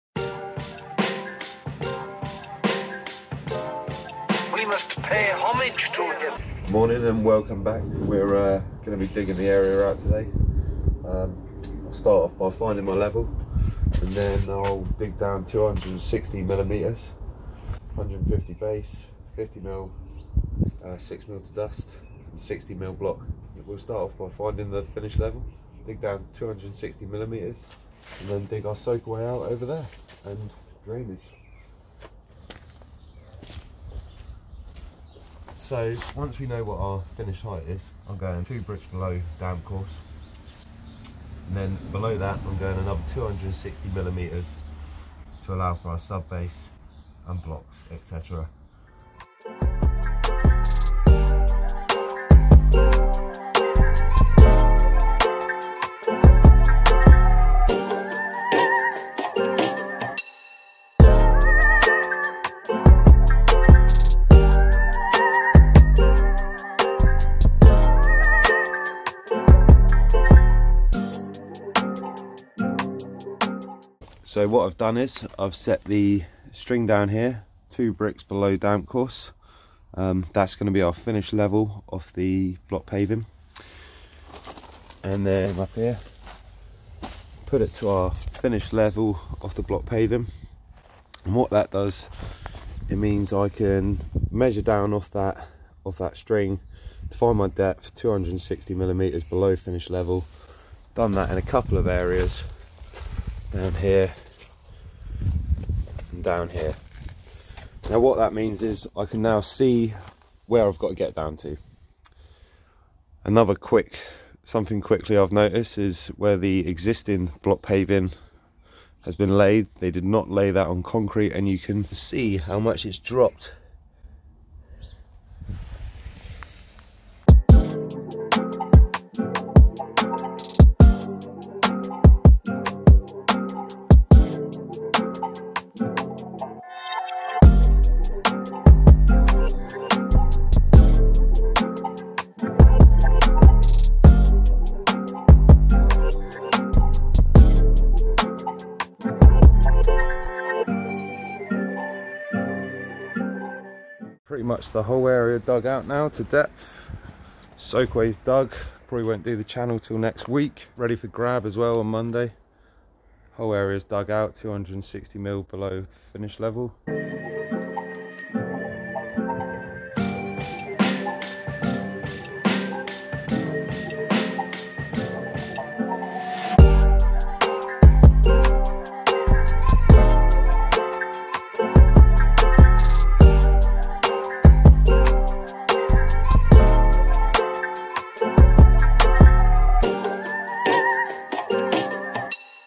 How to install a driveway - Digging the area out - Depths - Landscaping uk - VLOG02
the entire area is dug out 260mm below finished level.

Checkout one of our other platforms